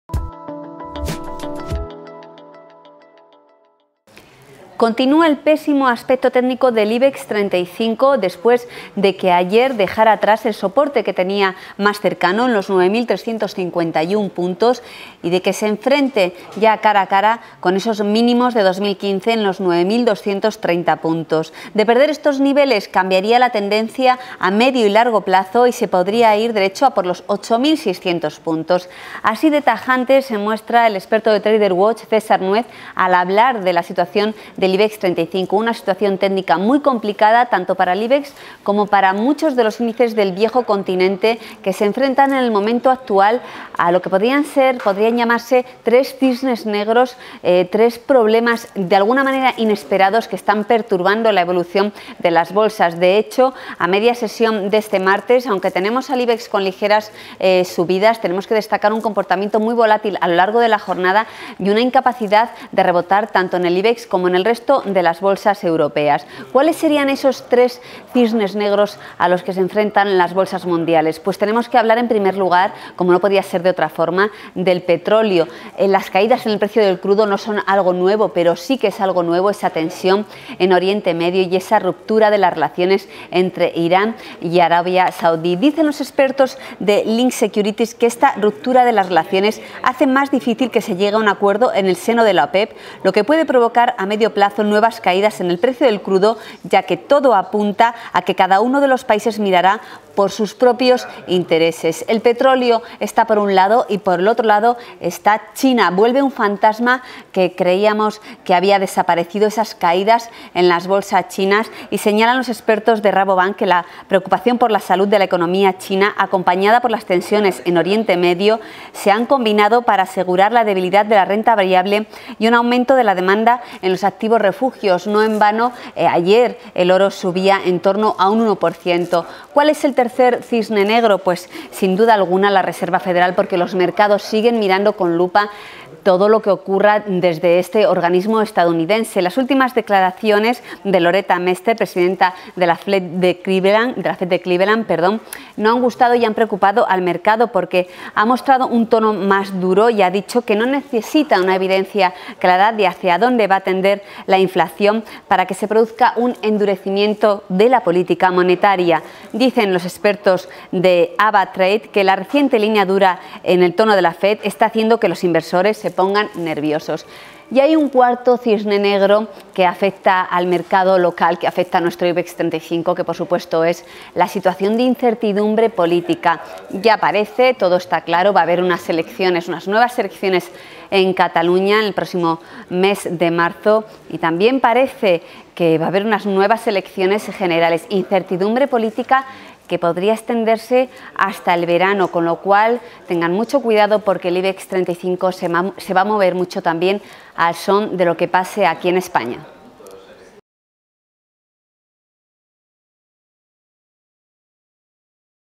La volatilidad del Ibex y los 4 cisnes negros a los que debe hacer frente
"Continúa el pésimo aspecto técnico del Ibex que ayer perdía el soporte clave del corto plazo que presenta en los 9.351 puntos, dejando una 'isla bajista', una figura muy bajista". Así de claro se muestra el experto de Trader Watch, César Nuez, al hablar del Ibex.